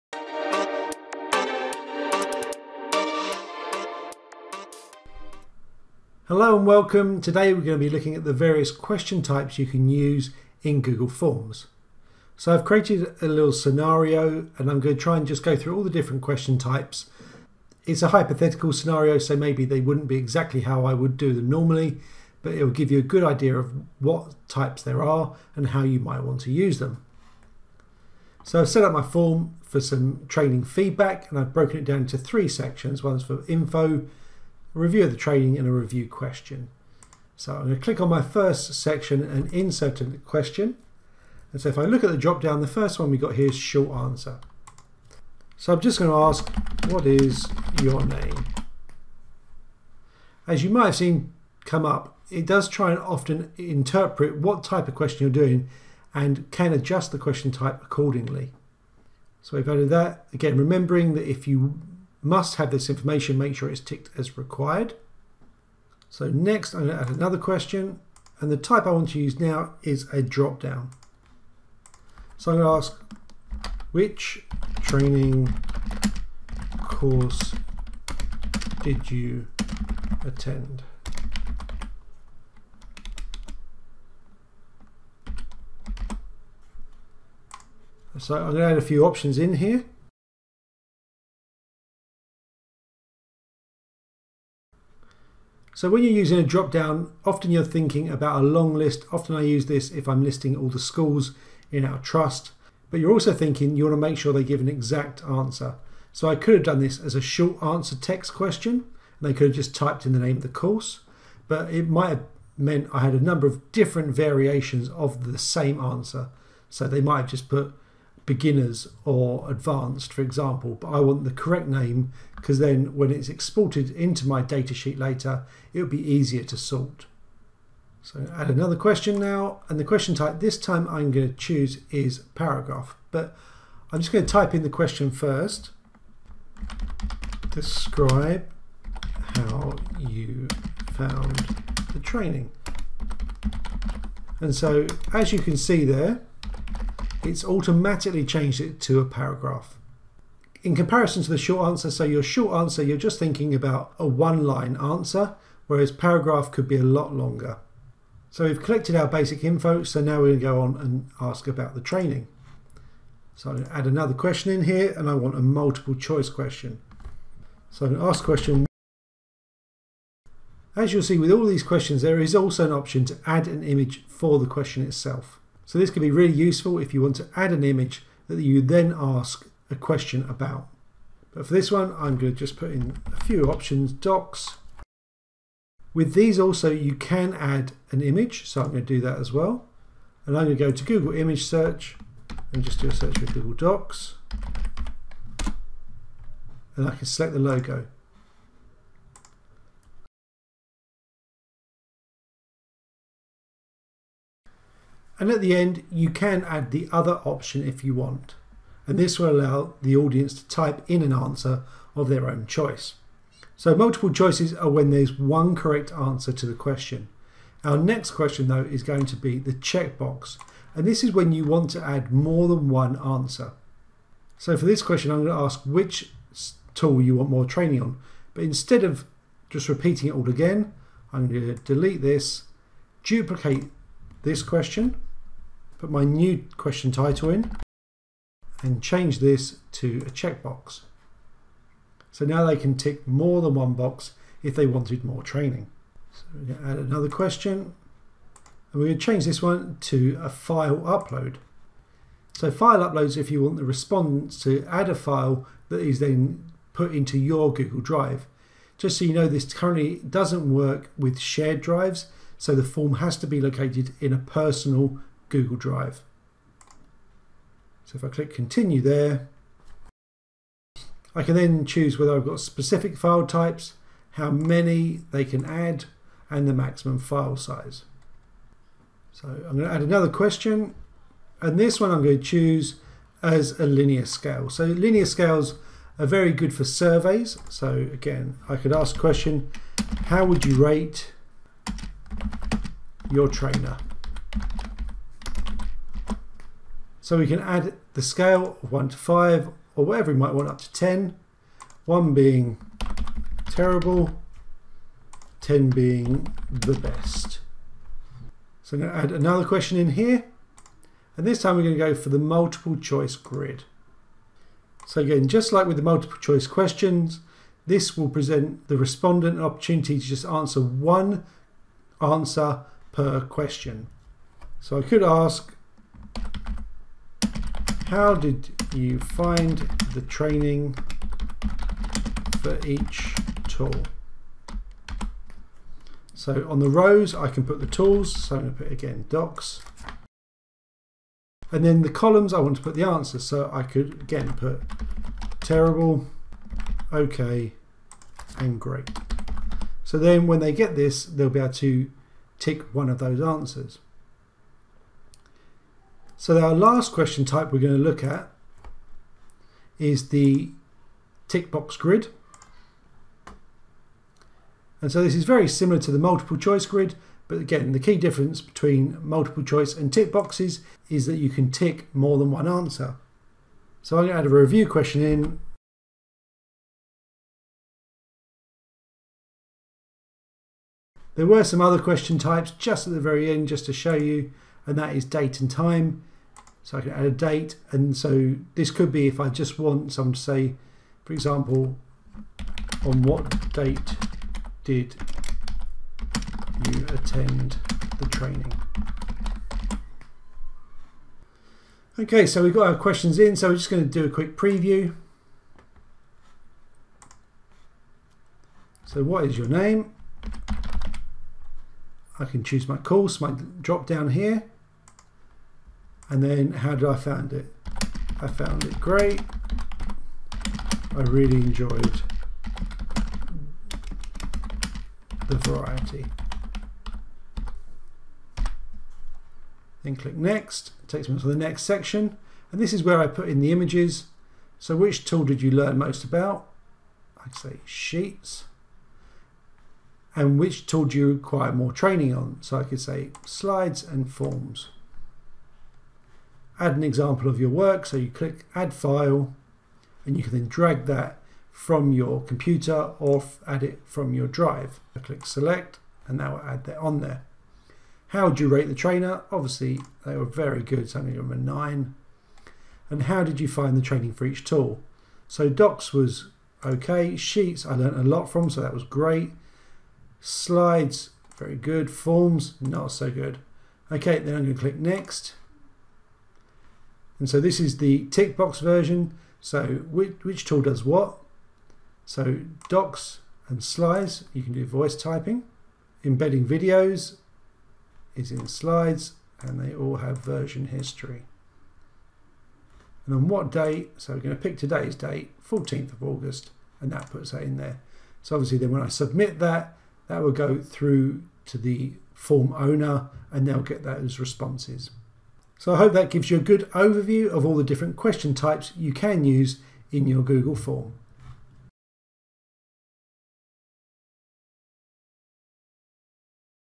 Understanding Google Forms question types
There are different question types you can use in Google Forms. Choosing the right type will help you get the information you need.